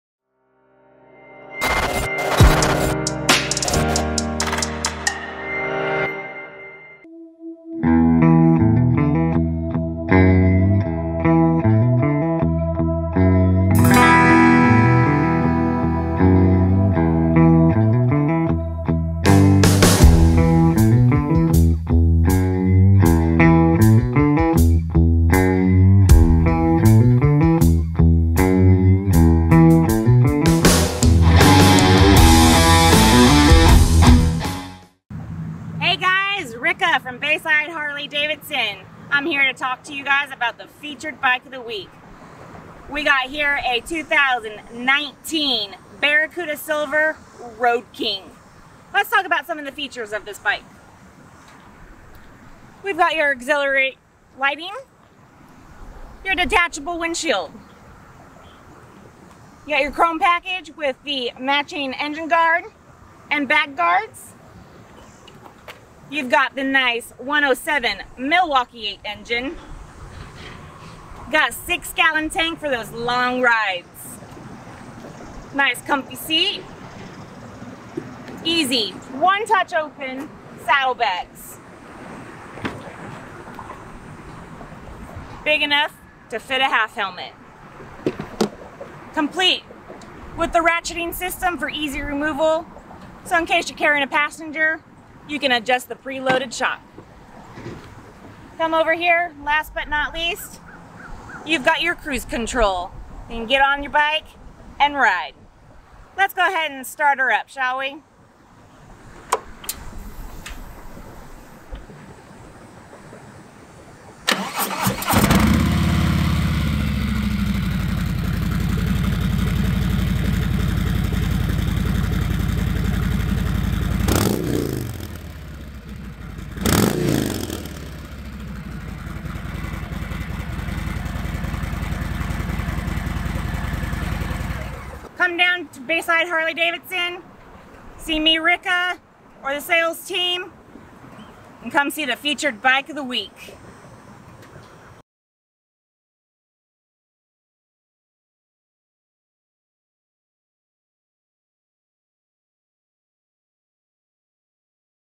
2019 Harley-Davidson Road King | Bayside's Bike Of The Week
Rikka here with Bayside Harley-Davidson in Portsmouth, VA here to show you this weeks Bike Of The Week! Today we feature a sweet 2019 Harley-Davidson Road King! This Bike features the amazing Milwaukee Eight 107 Engine, a clean Barracuda Silver Paint Job with the Chrome Package! It only has 4,000 miles on it, and was well taken care of!